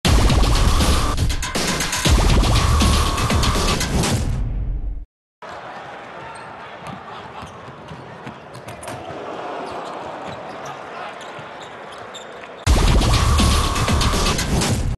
Majstorović with monster block on Kinsey
It would have been the dunk of the season... But Djordje Majstorović did not let Tarence Kinsey to finish his flight...